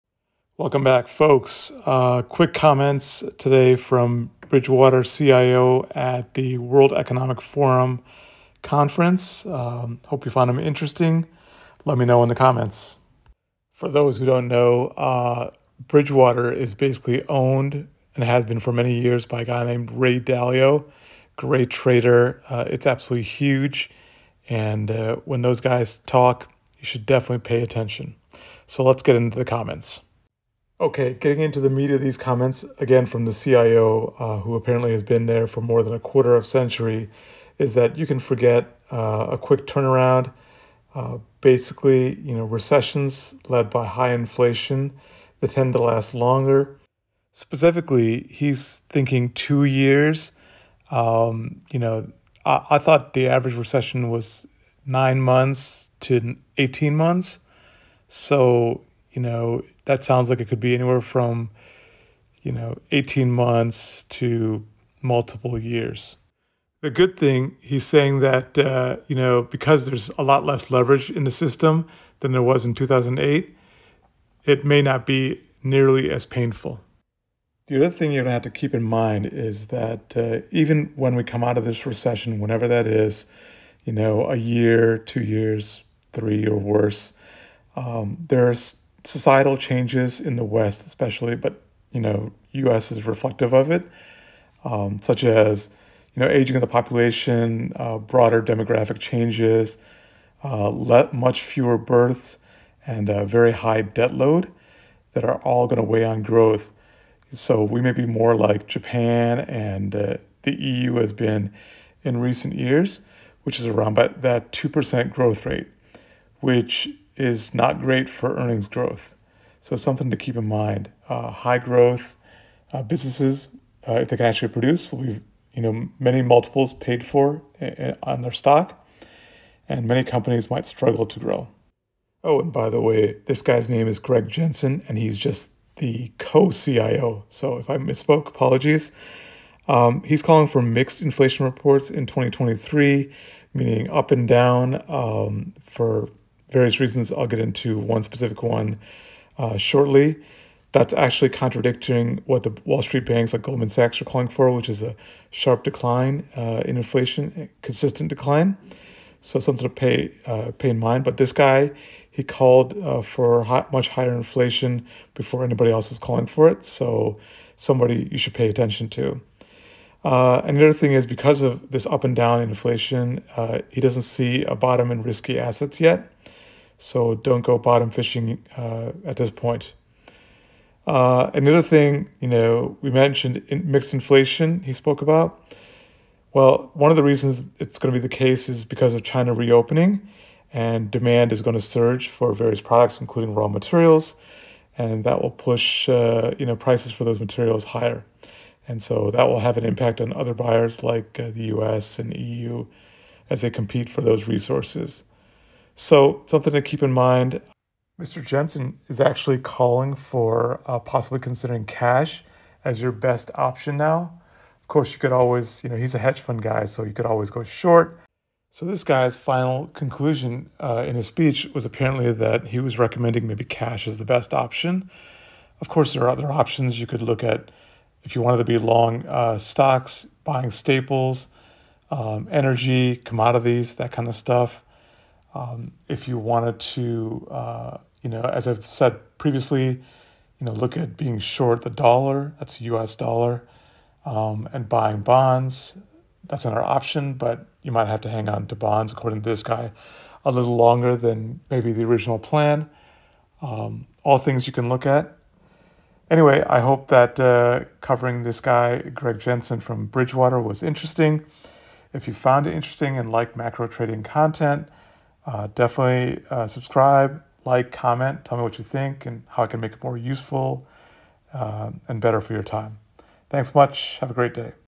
Bridgewater Says Cash is King
Bridgewater CIO speaks at WEF and says this recession will be longer and recovery inconsistent. China reopening will have an impact as will demographic changes when we stumble into recovery.

Sell USD
Buy bonds
Own staples and avoid risk assets unless short.

Bad news for big tech!